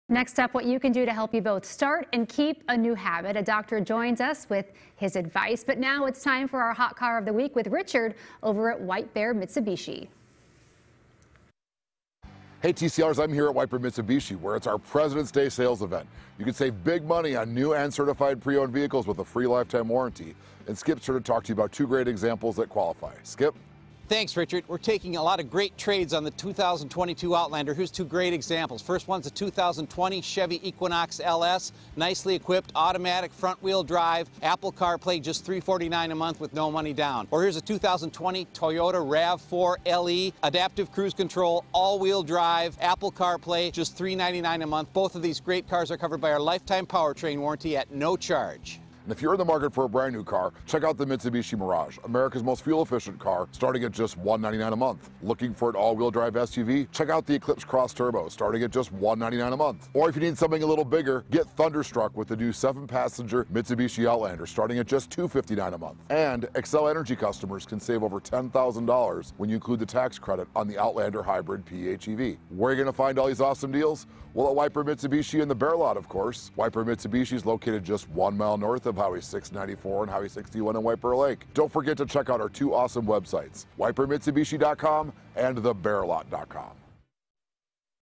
White Bear Mitsubishi Hot Car of the Week - February 23
It’s the President’s Day sales event at White Bear Mitsubishi and we have over 300 new and certified used vehicles in stock!

2020 Chevrolet Equinox LS
🔥 Buy for $26,996 OR
🔥 Finance for $349/month with no money down

2020 Toyota Rav4 LE
🔥 Buy for $29,996 OR
🔥 Finance for $399/month with no money down

Want a new ride? We have 300+ vehicles in stock now! Lease a NEW 2022 Mitsubishi Eclipse Cross ES S-AWC from $199/month. Looking for a seven-passenger four-wheel drive SUV? Check out the all-new 2022 Mitsubishi Outlander from just $259/month. If you’re looking for a hybrid, save up to $10,587 on the all-wheel drive Mitsubishi Outlander PHEV.